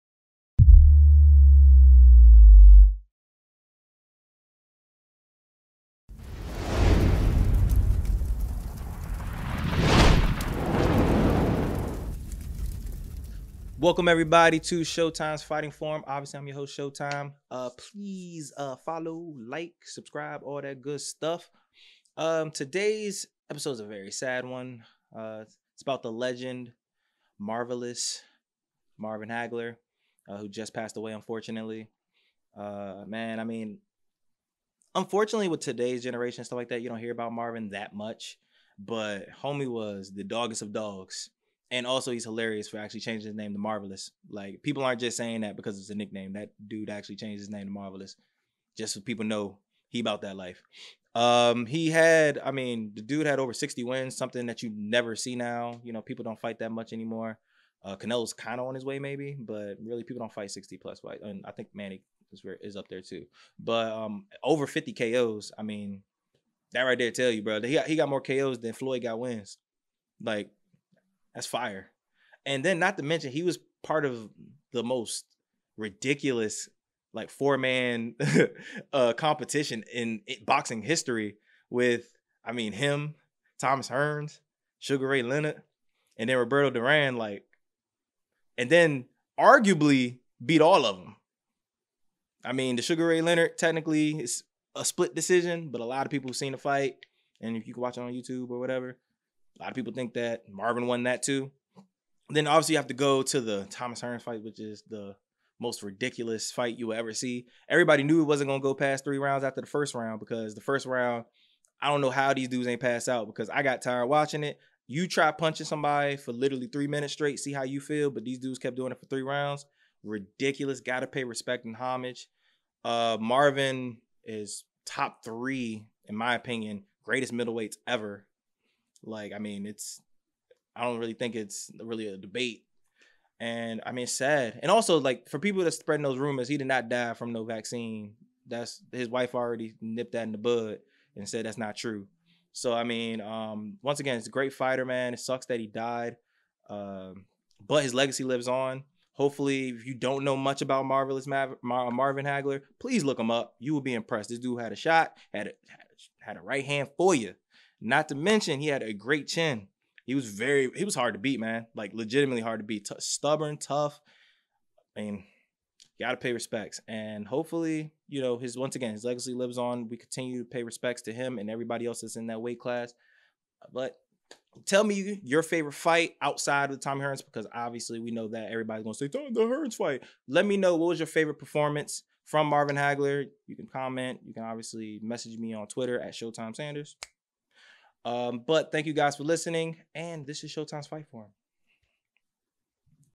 A Tribute to The Marvelous One: Showtime's Fight Forum
Rest in Peace to one of the Greatest Middleweight Boxers of All Time, Marvelous Marvin Hagler

Follow Showtime Sanders on Twitter and Instagram @ShowtimeSanders